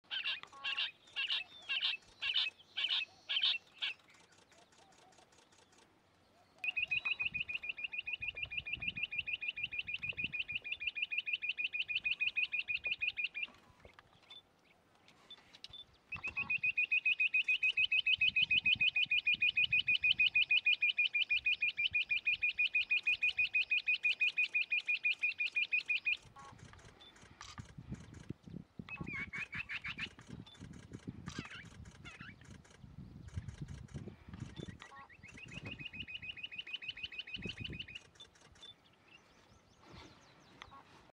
Call of Rock Bush-Quail I Indian bird Call I Dr. Chetna Ugale
Rock Bush-Quail
Perdicula argoondah
A squat and rotund bush-quail. Males have a dull chestnut forecrown and face, a thin white eyebrow over a dark brown eyeline that broadens behind the eye. Lightly barred brown above and boldly barred in black and white below. Females have a similar but duller head pattern with vinaceous underparts without barring. Note the slaty gray bill and orange legs. Found in pairs or family groups in dry rocky areas with scrub. Secretive, but its musical trill followed by rapidly ascending piping notes is often heard.
Source information: Ebird website

Video by - Dr. Chetna Ugale
Location: Kutchh Gujrat